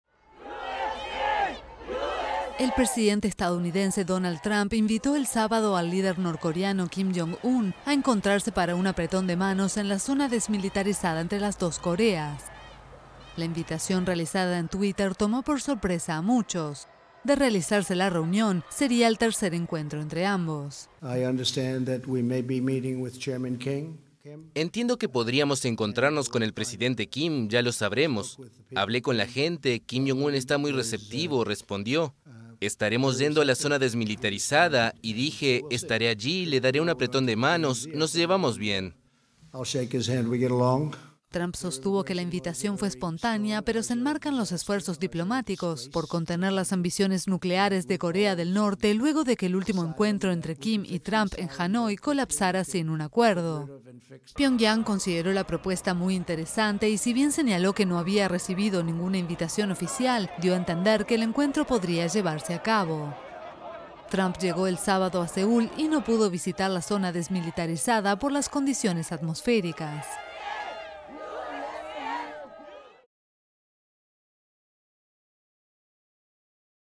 Donald Trump propone a Kim Jong-un una reunión en la frontera entre las dos Coreas
El presidente de Estados Unidos propuso al líder norcoreano, Kim Jong-un, una reunión en la zona desmilitarizada situada entre las dos Coreas.